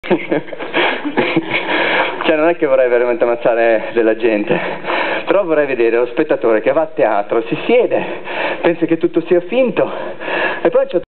ROMA - SHORT THEATRE
arts + opinions
ZONA ESTERNA - TEATRO
Motus
LET THE SUNSHINE IN (antigone)contest #1
ideazione e regia Enrico Casagrande & Daniela Nicolò
con Silvia Calderoni e Benno Steinegger
durata: 50'

Archivio  arts + opinions